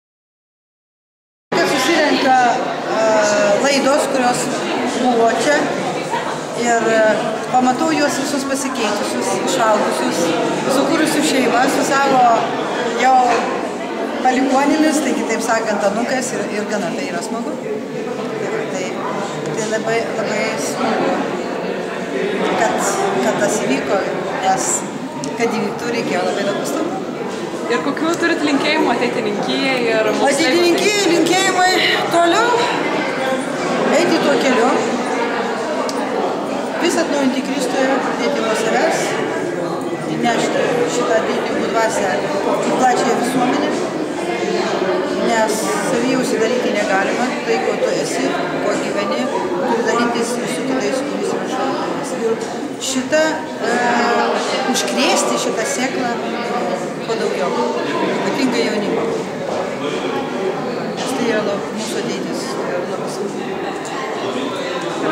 Interviu su Vida Zulonaite
MAS valdybos nariai pakalbino Vidą Zulonaitę per jos globojamos Šv. Kazimiero kuopos 25-ąjį gimtadienį